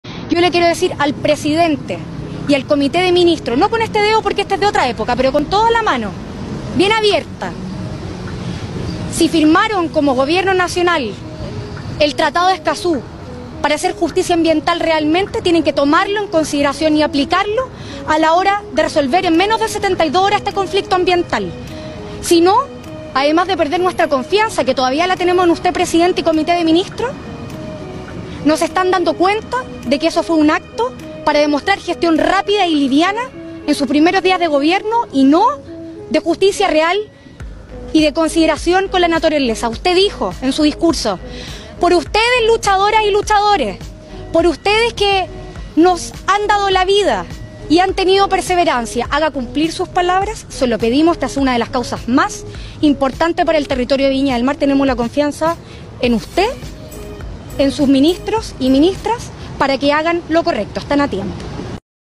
"Haga cumplir sus palabras": Alcaldesa Ripamonti da ultimátum a Boric por proyecto Las Salinas
"Haga cumplir sus palabras": Alcaldesa Ripamonti (FA) de Viña del Mar da ultimátum de 72 horas al gobierno de Boric para rechazar proyecto Las Salinas